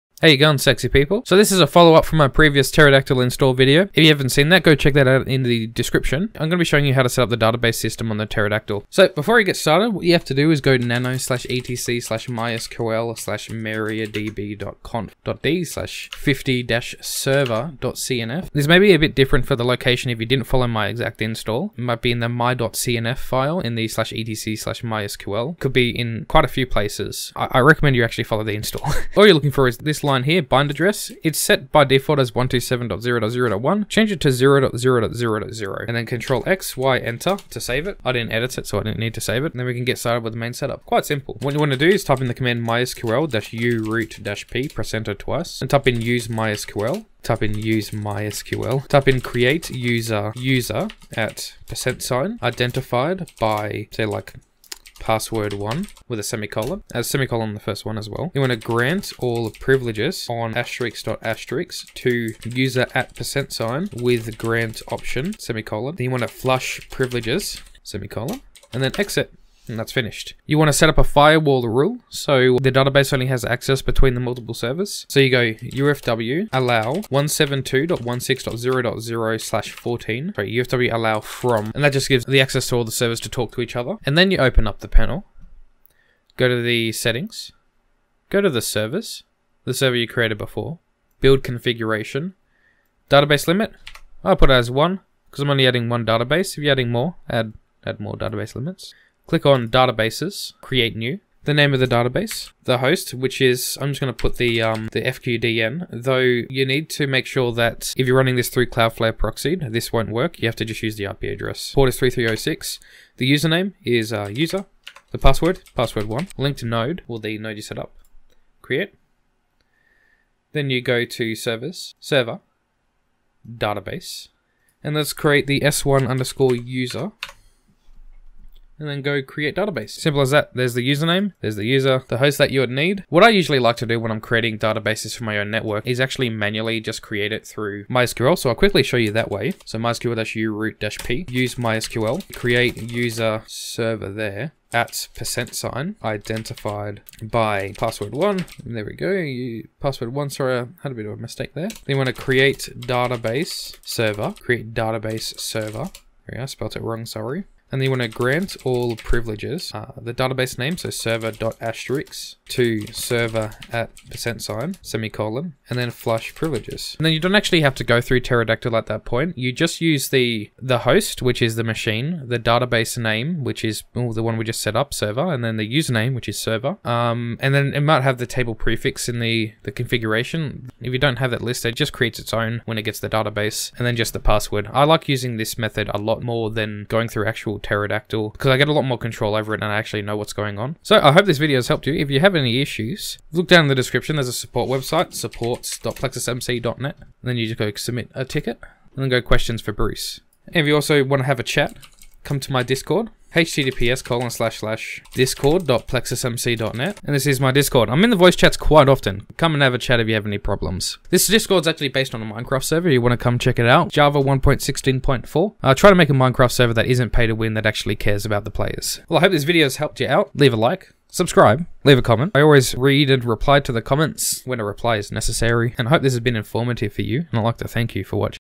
Just Fancy Text Files | Setting up Databases on Pterodactyl 2020 No Time Wasted
Hey peeps. This is a simple video explaining how to set up databases to be used on Pterodactyl. This can be used with multiple servers talking to each other or just singular servers, for hosting companies. These should be fairly secure as long as you have a good password. I missed a step in the video, check the Commands to set up, to see how to restart MySQL.

Minecraft Server:
play.plexusmc.net 1.16.4